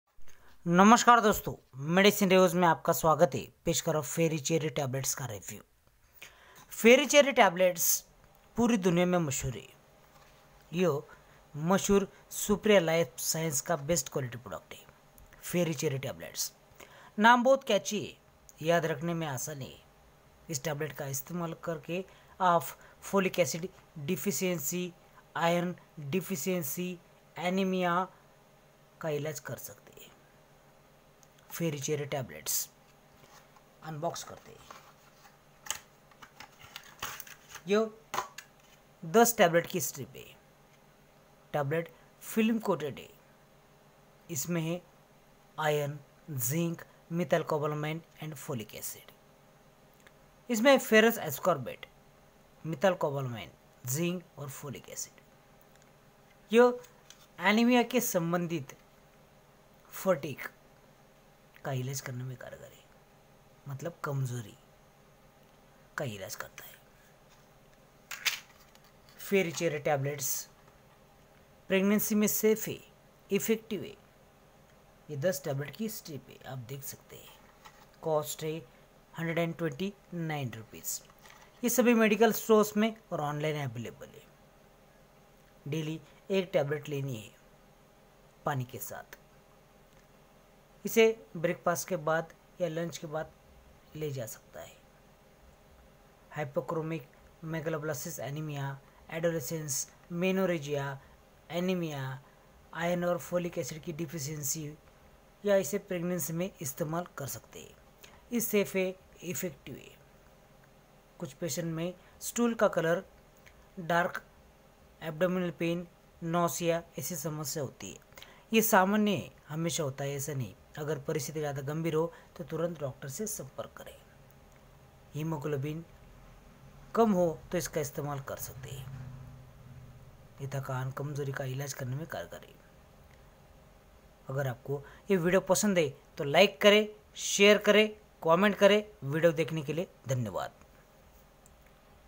Ferry Cherry Tablets review in Hindi
Ferry Cherry Tablets is best combination of Iron , Zinc ,Methylcobalamin and folic acid
Ferry Cherry Tablets is safe
Ferry Cherry Tablets is effective
Ferry Cherry Tablets is used in the treatment of tiredness and fatigue
Ferry Cherry Tablets is increases blood level as well as Hb count online-medicine , medicine,Medicine Reviews Instagram id vijaylahore4262